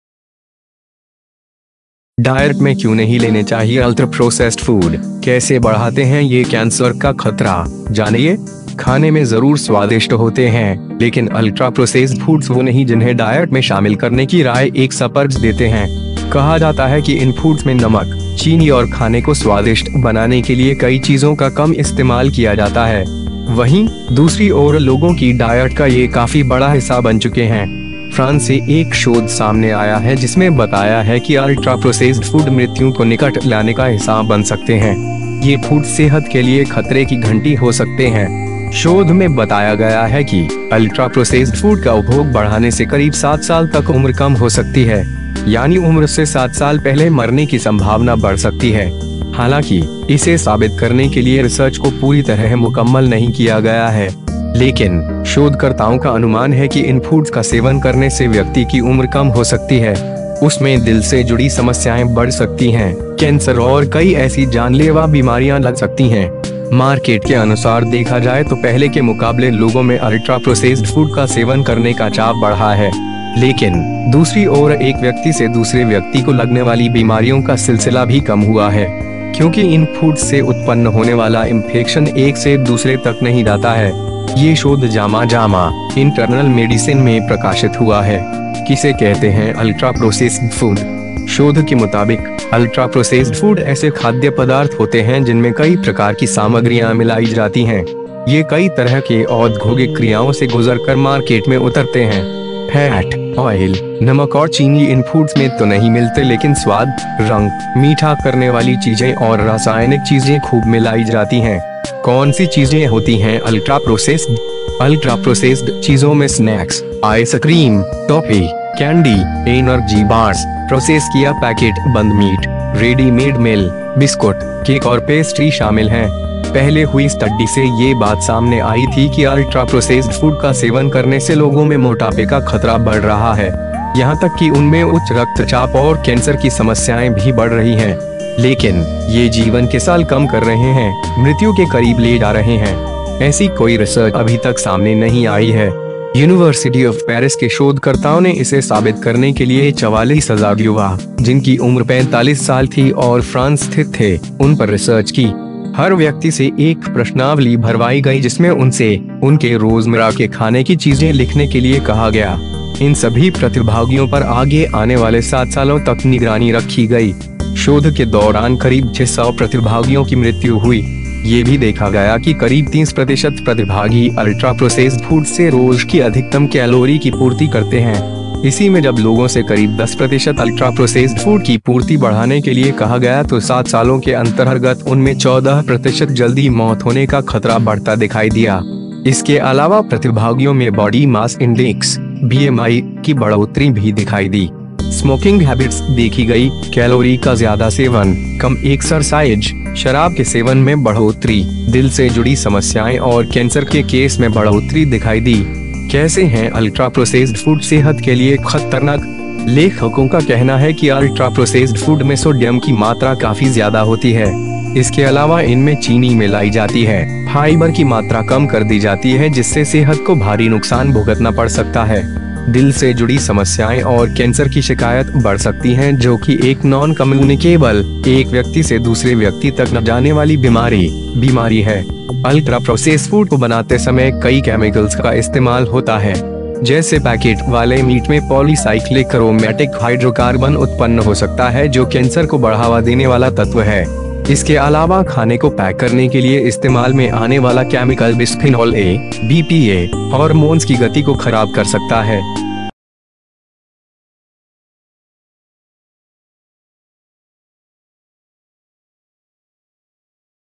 डायट में क्यूं नहीं लेने चाहिए Ultra processed Food, कैसे बढ़ाते हैं ये Cancer का खतरा, जानिए
खाने में जरूर स्वादिष्ट होते हैं, लेकिन अल्ट्रा-प्रोसेस्ड फूड्स वो नहीं जिन्हें डायट में शामिल करने की राय एक्सपर्ट्स देते हैं. कहा जाता है कि इन फूड्स में नमक, चीनी और खाने को स्वादिष्ट बनाने के लिए कई चीजों का कम इस्तेमाल किया जाता है. वहीं, दूसरी ओर लोगों की डायट का ये काफी बड़ा हिस्सा बन चुके हैं.

फ्रांस से एक शोध सामने आया है जिसमें बताया है कि अल्ट्रा-प्रोसेस्ड फूड मृत्यू को निकट लाने का हिस्सा बन सकते हैं. ये फूड्स सेहत के लिए खतरे की घंटी हो सकते हैं.

शोध में बताया गया है कि, अल्ट्रा-प्रोसेस्ड फूड का उपभोग बढ़ाने से करीब सात साल तक उम्र कम हो सकती है. यानी उम्र से सात साल पहले मरने की संभावना बढ़ सकती है. हालांकि, इसे साबित करने के लिए रिसर्च को पूरी तरह मुक़म्मल नहीं किया गया है. लेकिन, शोधकर्ताओं का अनुमान है कि इन फूड्स का सेवन करने से व्यक्ति की उम्र कम हो सकती है. उसमें दिल से जुड़ी समस्याएं बढ़ सकती हैं, कैंसर और कई ऐसी जानलेवा बीमारियां लग सकती हैं.

मार्केट के अनुसार देखा जाए तो पहले के मुक़ाबले लोगों में अल्ट्रा-प्रोसेस्ड फूड का सेवन करने का चाव बढ़ा है. लेकिन, दूसरी ओर एक व्यक्ति से दूसरे व्यक्ति को लगने वाली बीमारियों का सिलसिला भी कम हुआ है. क्योंकि इन फूड्स से उत्पन्न होने वाला इंफेक्शन एक से दूसरे तक नहीं जाता है. ये शोध JAMA (जामा) इंटरनल मेडिसिन में प्रकाशित हुआ है.

किसे कहते हैं अल्ट्रा-प्रोसेस्ड फूड?
शोध के मुताबिक, अल्ट्रा-प्रोसेस्ड फूड ऐसे खाद्य पदार्थ होते हैं जिनमें कई प्रकार की सामग्रियां मिलाई जाती हैं. ये कई तरह के औद्घोगिक क्रियाओं से गुज़रकर मार्केट में उतरते हैं. फैट, ऑयल, नमक और चीनी इन फूड्स में तो नहीं मिलते लेकिन स्वाद, रंग, मीठा करने वाली चीजें और रासायनिक चीजें खूब मिलाई जाती हैं.

कौन-सी चीजें होती हैं अल्ट्रा-प्रोसेस्ड?
अल्ट्रा-प्रोसेस्ड चीजों में स्नैक्स, आइसक्रीम, टॉफी, कैंडी, एनर्जी बार्स, प्रोसेस किया पैकेट बंद मीट, रेडी-मेड मील, बिस्कुट, केक और पेस्ट्री शामिल हैं. पहले हुई स्टडी से ये बात सामने आई थी कि अल्ट्रा-प्रोसेस्ड फूड का सेवन करने से लोगों में मोटापे का खतरा बढ़ रहा है. यहां तक कि उनमें उच्च रक्तचाप और कैंसर की समस्याएं भी बढ़ रही हैं.

लेकिन, ये जीवन के साल कम कर रहे हैं, मृत्यू के करीब ले जा रहे हैं. ऐसी कोई रिसर्च अभी तक सामने नहीं आई है.

यूनिवर्सिटी ऑफ पैरिस के शोधकर्ताओं ने इसे साबित करने के लिए 44 हजार युवा, जिनकी उम्र 45 साल थी और फ्रांस स्थित थे, उन पर रिसर्च की. हर व्यक्ति से एक प्रश्नावली भरवाई गई जिसमें उनसे, उनके रोज़मर्रा के खाने की चीजें लिखने के लिए कहा गया. इन सभी प्रतिभागियों पर आगे आने वाले सात सालों तक निगरानी रखी गई. शोध के दौरान करीब 600 प्रतिभागियों की मृत्यू हुई. ये भी देखा गया कि करीब 30 प्रतिशत प्रतिभागी अल्ट्रा-प्रोसेस्ड फूड्स से रोज़ की अधिकतम कैलोरी की पूर्ति करते हैं. इसी में जब लोगों से करीब 10 प्रतिशत अल्ट्रा-प्रोसेस्ड फूड की पूर्ति बढ़ाने के लिए कहा गया तो सात सालों के अंतरर्गत उनमें 14 प्रतिशत जल्दी मौत होने का खतरा बढ़ता दिखाई दिया.

इसके अलावा प्रतिभागियों में बॉडी मास इंडेक्स (बीएमआई) की बढ़ोतरी भी दिखाई दी. स्मोकिंग हैबिट्स देखी गई, कैलोरी का ज्यादा सेवन, कम एक्सरसाइज, शराब के सेवन में बढ़ोतरी, दिल से जुड़ी समस्याएं और कैंसर के केस में बढ़ोतरी दिखाई दी.

कैसे हैं अल्ट्रा-प्रोसेस्ड फूड सेहत के लिए खतरनाक?
लेखकों का कहना है कि अल्ट्रा-प्रोसेस्ड फूड में सोडियम की मात्रा काफी ज्यादा होती है. इसके अलावा इनमें चीनी मिलाई जाती है, फाइबर की मात्रा कम कर दी जाती है जिससे सेहत को भारी नुकसान भुगतना पड़ सकता है. दिल से जुड़ी समस्याएं और कैंसर की शिकायत बढ़ सकती हैं जोकि एक नॉन-कम्यूनिकेबल (एक व्यक्ति से दूसरे व्यक्ति तक न जाने वाली बीमारी) बीमारी है.

अल्ट्रा-प्रोसेस फूड को बनाते समय कई कैमिकल्स का इस्तेमाल होता है. जैसे पैकेट वाले मीट में पॉलीसाइक्लिक अरोमैटिक हाइड्रोकार्बन उत्पन्न हो सकता है जो कैंसर को बढ़ावा देने वाला तत्व है. इसके अलावा खाने को पैक करने के लिए इस्तेमाल में आने वाला कैमिकल बिसफिनॉल ए (बीपीए), हॉर्मोन्स की गति को खराब कर सकता है.